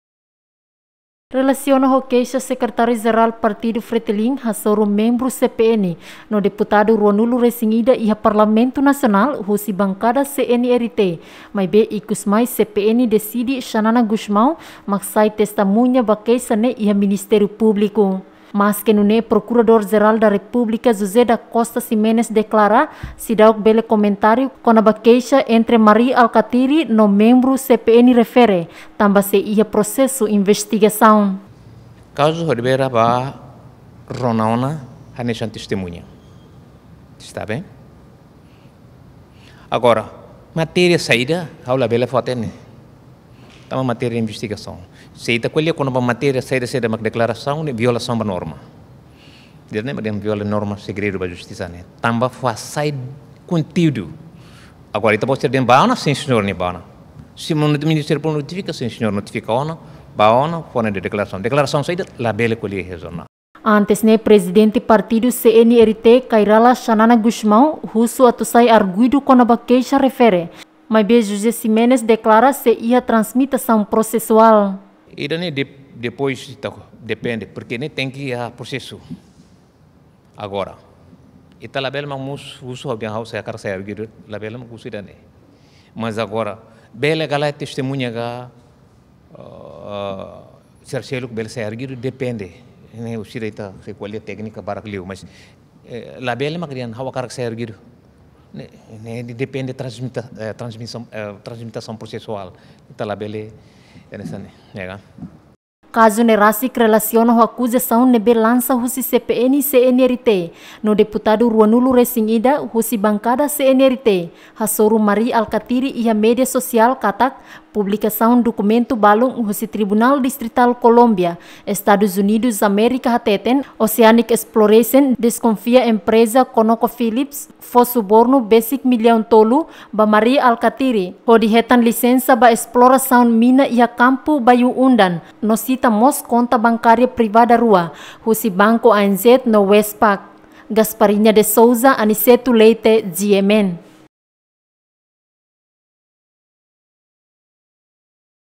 MP seidauk bele pronunsia keixa “Mari Alkatiri”
Ministériu Públiku seidauk bele pronunsia keixa SekJer FRETILIN “Mari Alkatiri” hasoru membru CPN no Deputadu 21 husi Bankada CNRT